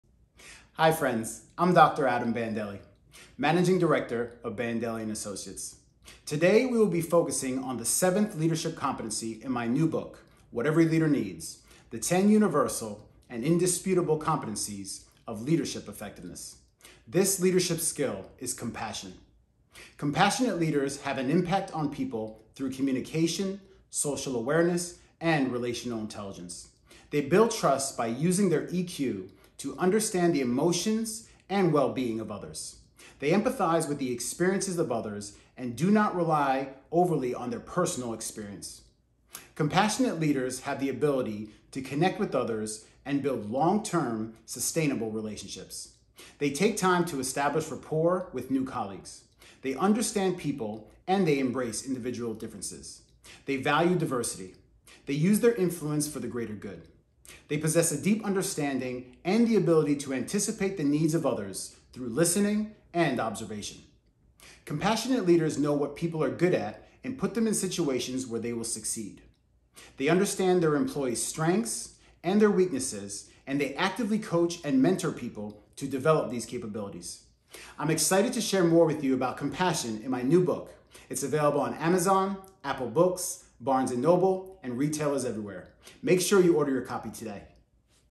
Leadership in 60 Seconds | What Every Leader Needs: Session 8 – COMPASSION | Adam Bandelli, Ph.D.
Dr. Adam C. Bandelli, Managing Director of Bandelli & Associates, continues the series on his new book What Every Leader Needs: The Ten Universal and Indisputable Competencies of Leadership Effectiveness. Today, he focuses on the seventh leadership competency in the book. This skill is COMPASSION. Compassionate leaders have an impact on people through communication, social awareness, and relational intelligence. They have the ability to connect with others and build long-term sustainable relationships. They take time to establish rapport with new people. They understand others by using their EQ, actively listening, being curious, and showing empathy towards others. The value diversity and embrace individual differences. They build trust by honoring their commitments and showing up consistently for others. The cultivate influence by having a positive, lasting impact on those around them. I’m excited to share more with you about COMPASSION in my new book. The audiobook was also just recently released on Audible!

Leadership Matters. Without It, People Fail.